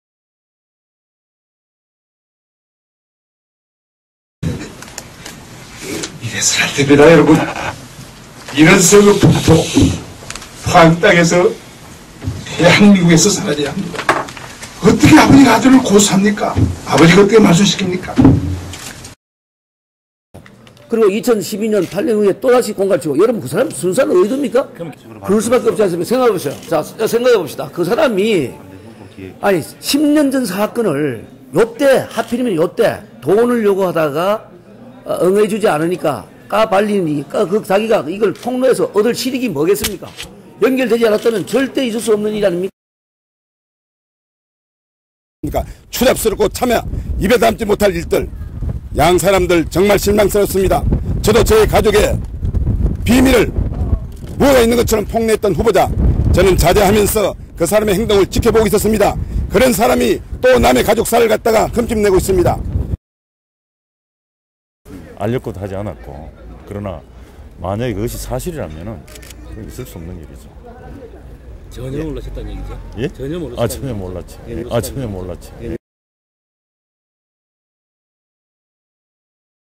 포항남.울릉 막판 과열 혼탁선거
19대 국회의원선거가 막판 혼탁,과열선거로 치닫고 있다. 가족경력,성추행,협박 등 온갖 추측성 폭로가 자행되고 있다. 이는 차후에도 선거법위반,명예회손등 법정시비 공방이 에상되고 있다.